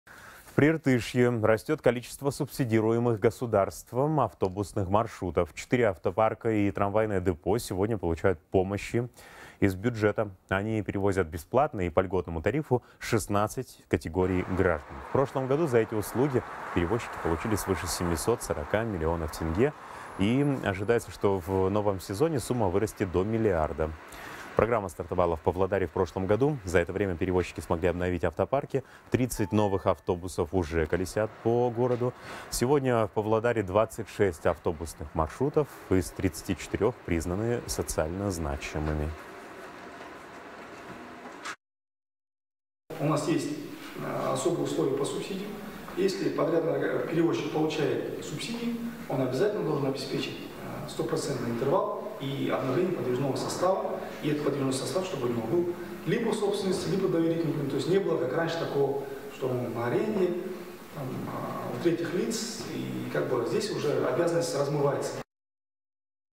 В Прииртышье растет число субсидируемых автобусных маршрутов
В Прииртышье растёт количество субсидируемых государством автобусных маршрутов, сообщает корреспондент «Хабар 24». Четыре автопарка и трамвайное депо сегодня получают помощь из бюджета. Они перевозят бесплатно и по льготному тарифу 16 категорий граждан.